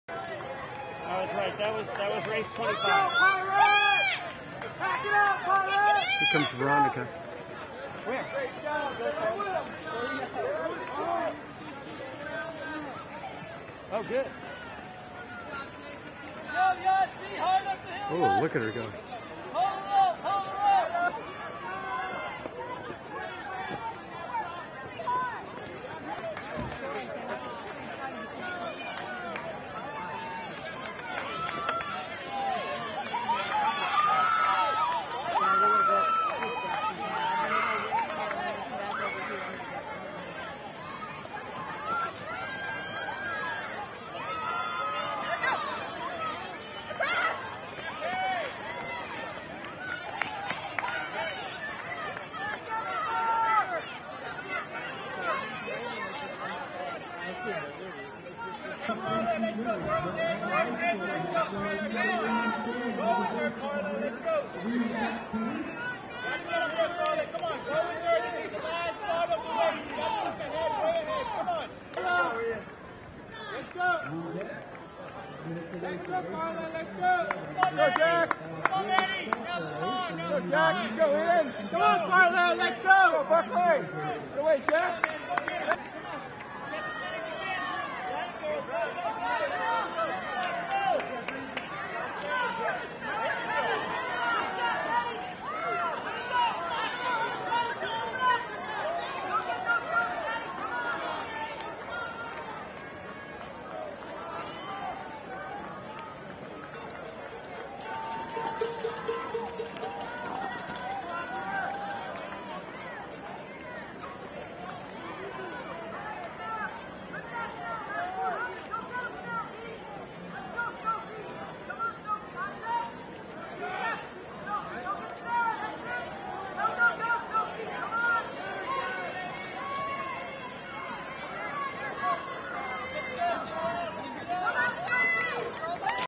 2010 SMHS XC - Frosh Girls at Mt SAC Invitational
San Marino's girls race the region's other Division 4 freshman cross country teams at the Mt. SAC Invitational on October 22, 2010.